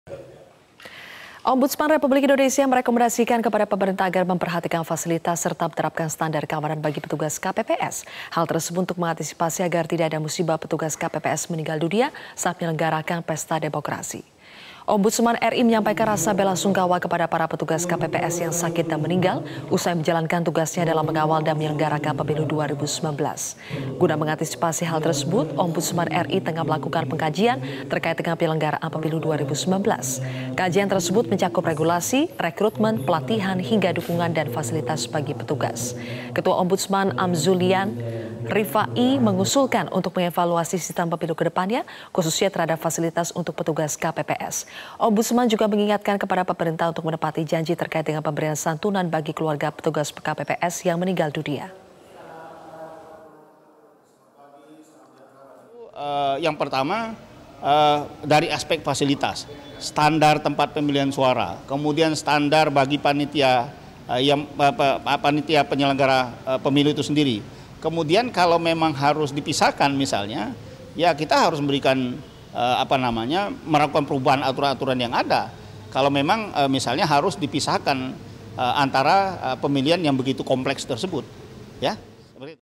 Ombudsman Kaji Penyelenggaraan Pemilu 2019
Ombudsman RI merekomendasikan kepada pemerintah agar memperhatikan fasilitas serta menerapkan standar keamanan bagi petugas KPPS. Hal tersebut untuk mengantisipasi agar tidak ada musibah petugas KPPS meninggal dunia saat menyelenggarakan pesta demokrasi.